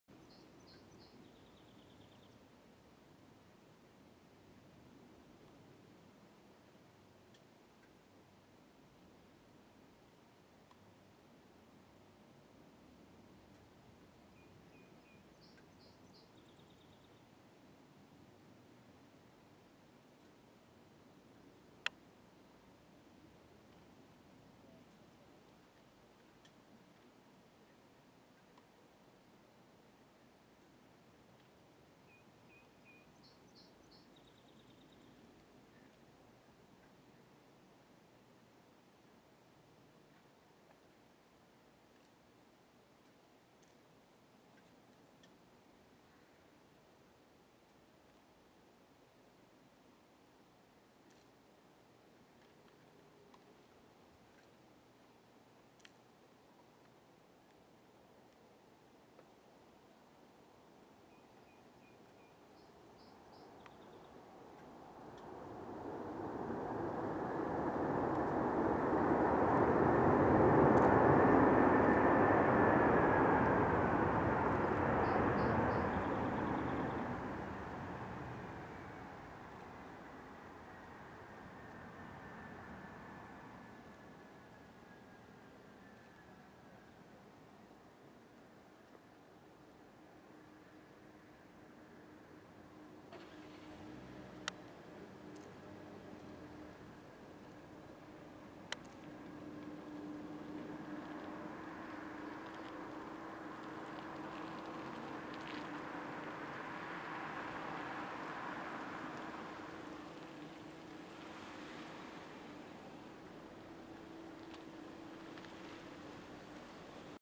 Swiss Airbus A330 Landing in Montreal YUL
Swiss (LX) Airbus A330 landing in Montreal (YUL - CYUL) on runway 06L. Registration HB-IQJ